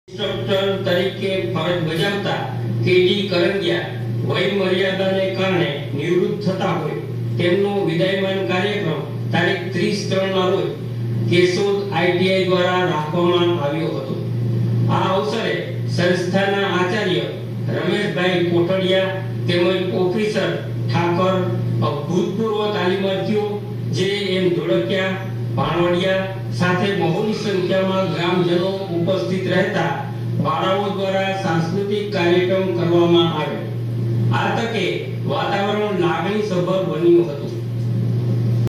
Ajab KVK
Retirement party of Mr K D Karangiya
Welcome to Mr Jiendra Dholakiya